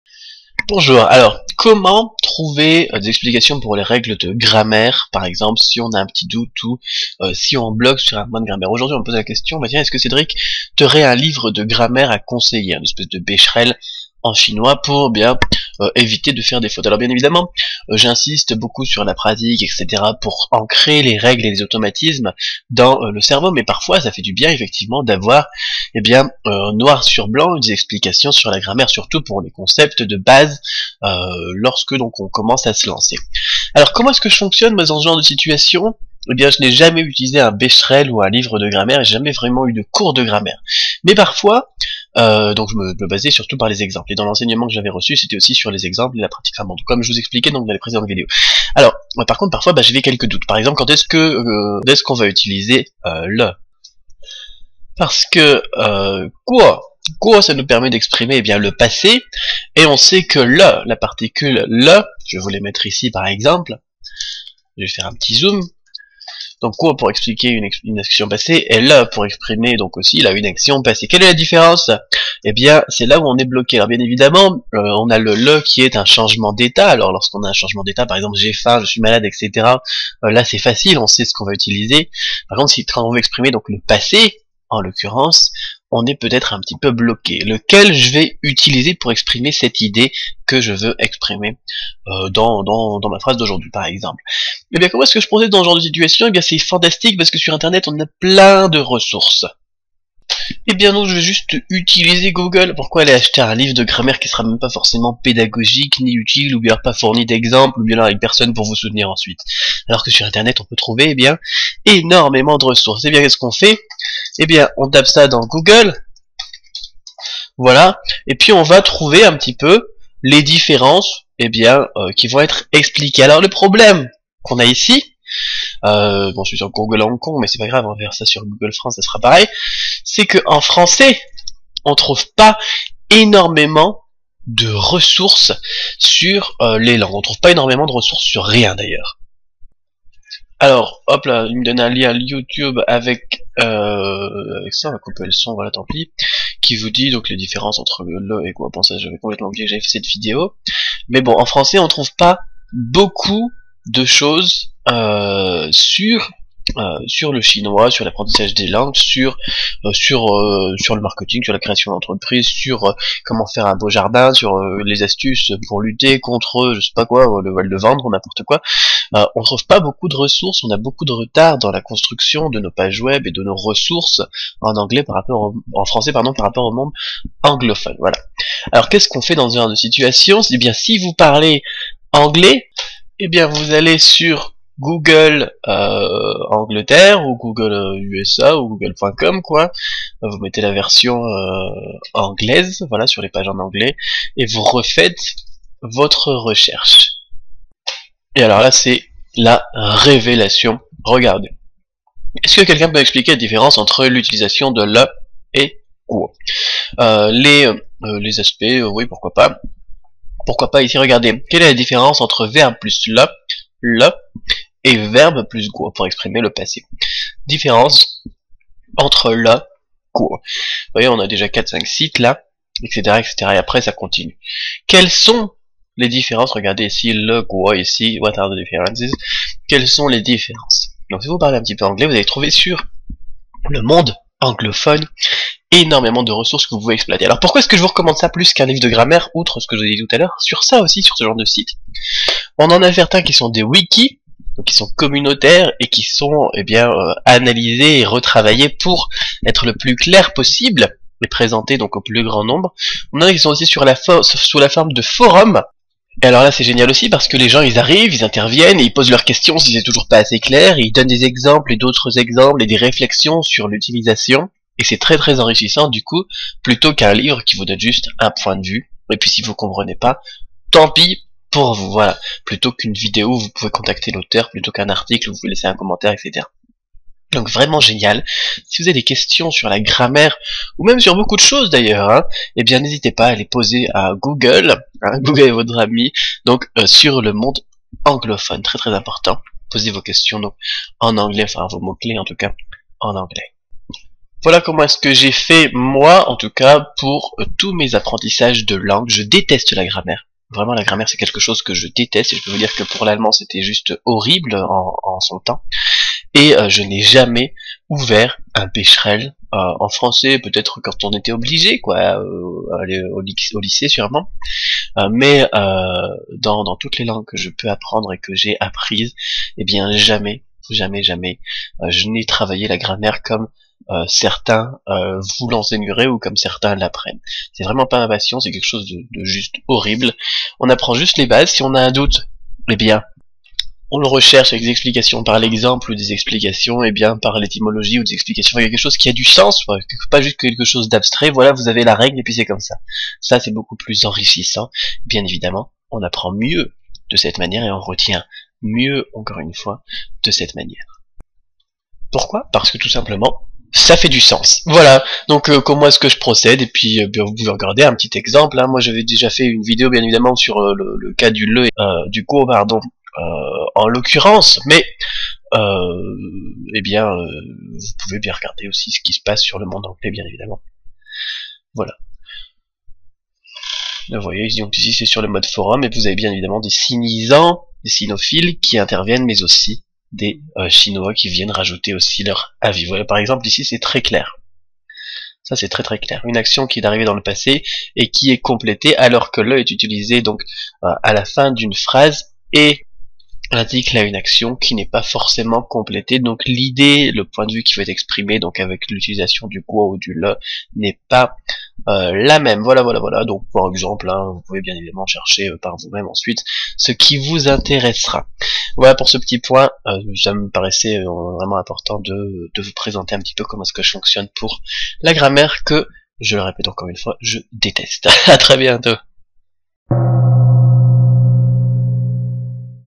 Outil : la meilleure méthode pour comprendre une règle de grammaire en Chinois
La grammaire en chinois, quel outil utiliser pour la comprendre.

Un doute sur une règle de grammaire (structure, ordre des mots) ou sur quel terme de vocabulaire utiliser entre deux mots qui semblent avoir le même sens?

Voici ma méthode